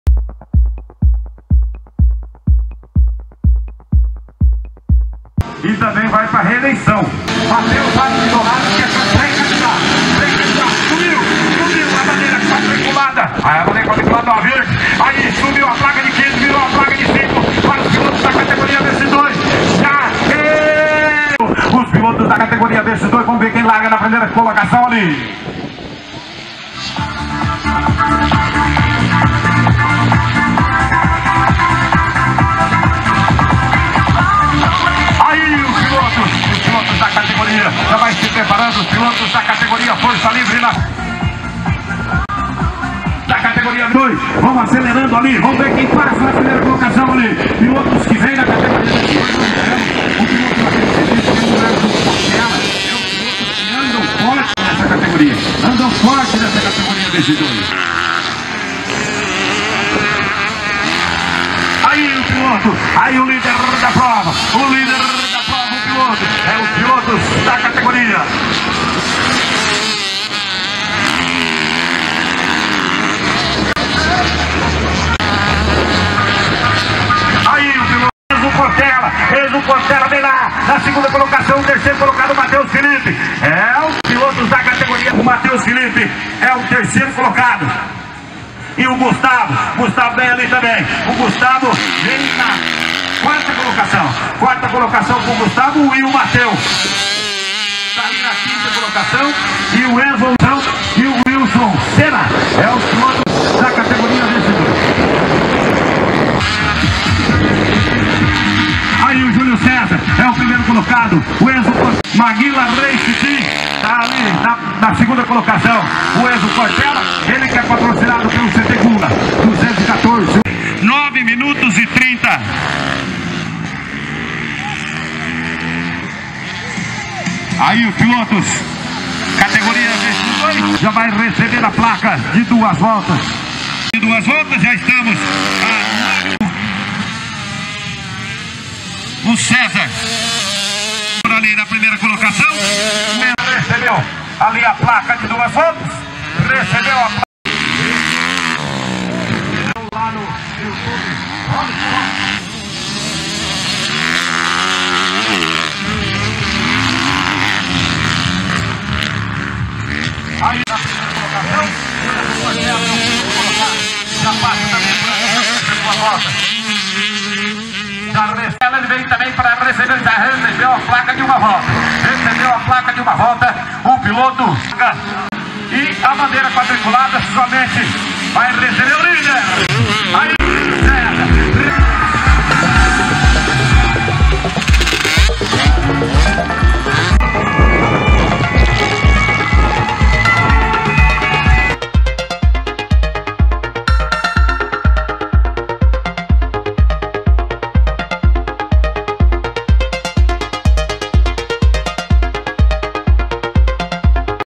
VX2JR na 5º etapa da Copa Sulmems de Velocross 2024 em Caarapó
Bateria disputada no domingo, 09 de junho de 2024

Acesse o blog @TVHoleshot